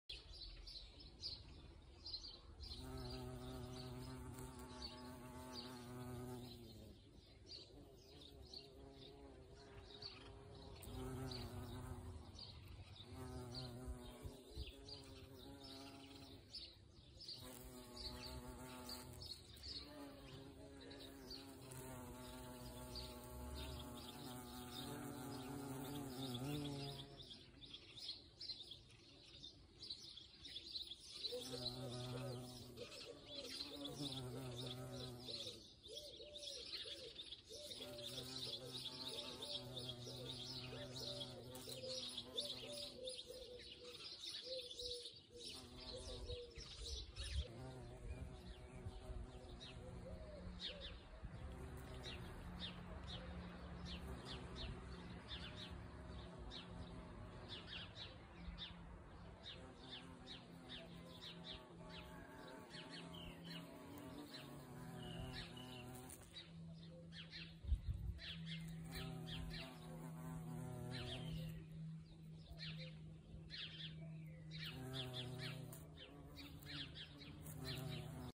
Le Xylocope est un addict à la sauge sclarée
La sauge sclarée "Salvia sclarea", une aromatique, herbacée, bisannuelle, une source de nourriture, une vraie mangeoire à insectes auxiliaires et abeilles.
Plante mellifère, floraison de juin à octobre
Le xylocope violet (Xylocopa violacea)
Abeille noire, xylocope, abeille charpentière, impressionnant, mais inoffensif pour l’homme.

Parc Naturel de la Haute Vallée de Chevreuse
Plante mellifère